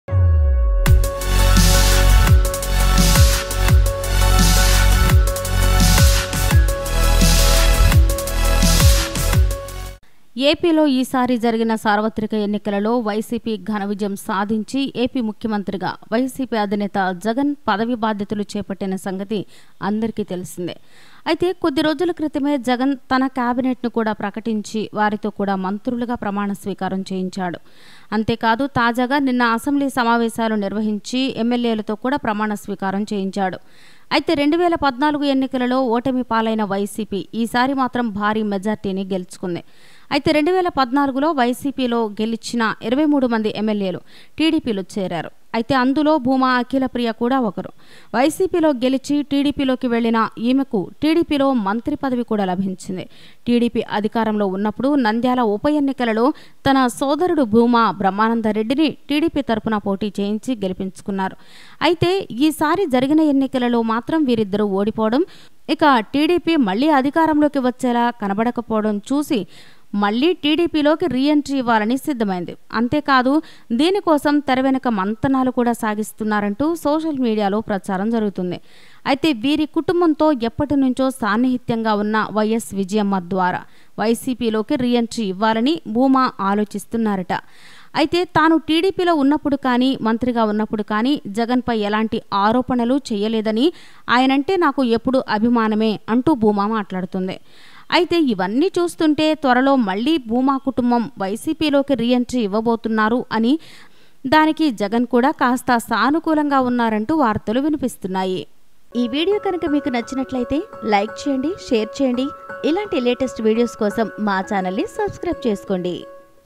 Line Cleared to Bhuma Akhila Priya to join YSRCP | Huge shock to TDP
Watch,  Line Cleared to Bhuma Akhila Priya to join YSRCP | Huge shock to TDP